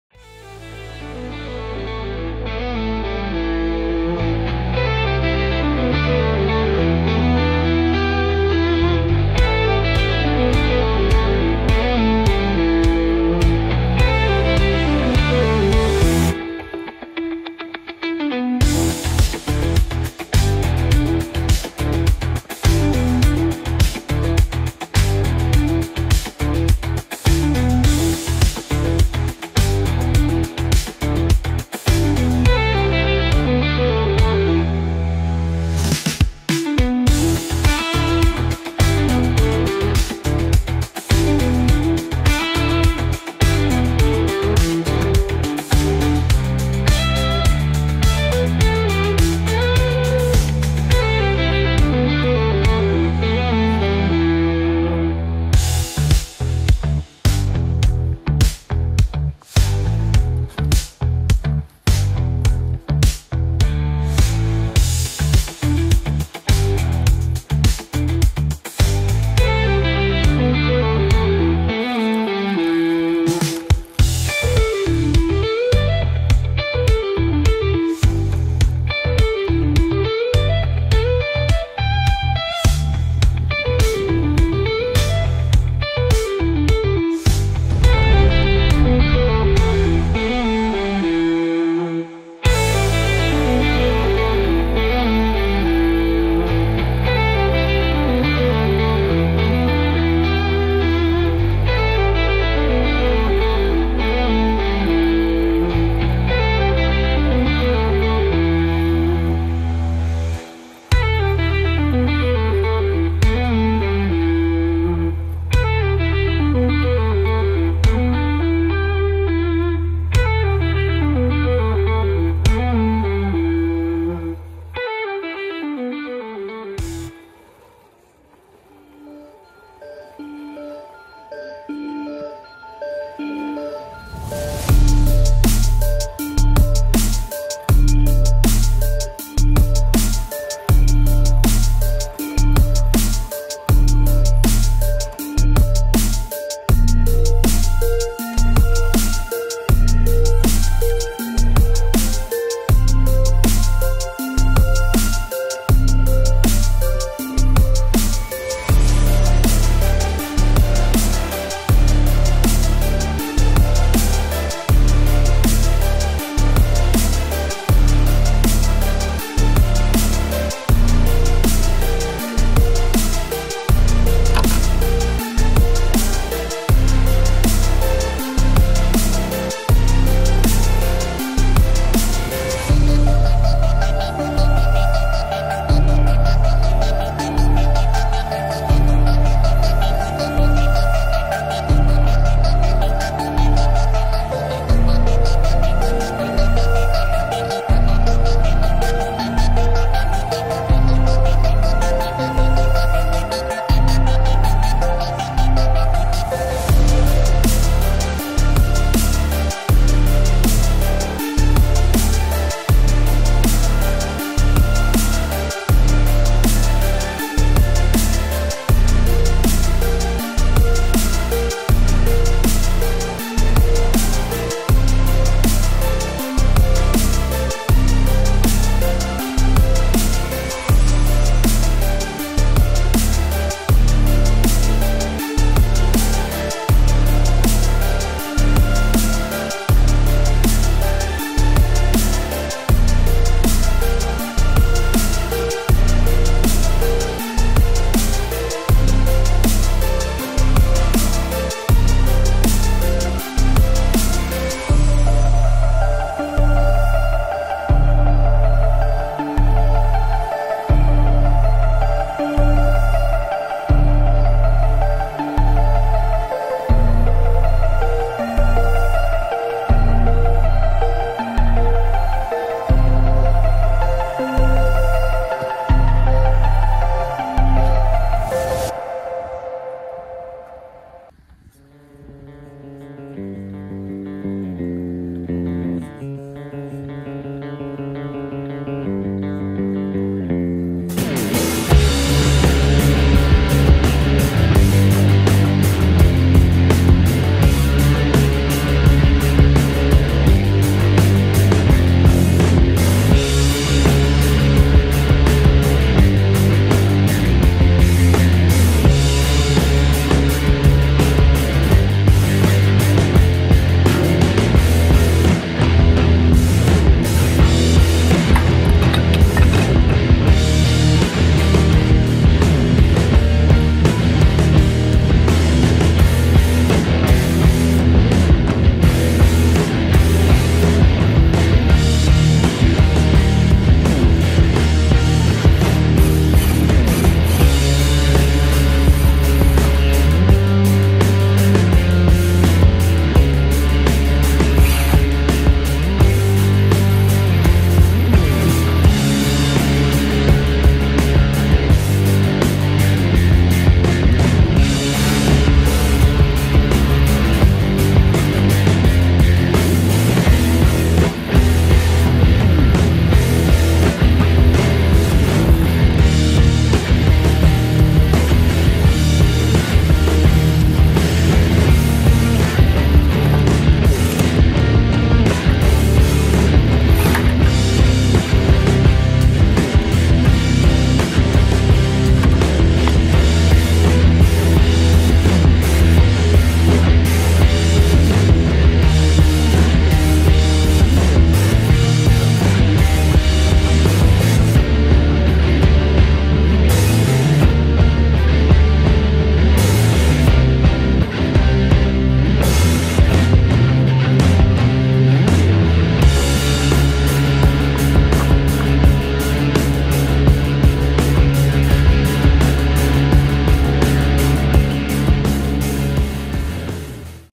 Expanding the crawler course in The Pit, with the tallest obstacle yet!
Adding to the crawler course here in The Pit, last week I was working on placing rock on the ground, this week we’re building up in the trees. Still have a long ways to go before calling it “done”, but I couldn’t resist giving it a test run and sharing it with ya.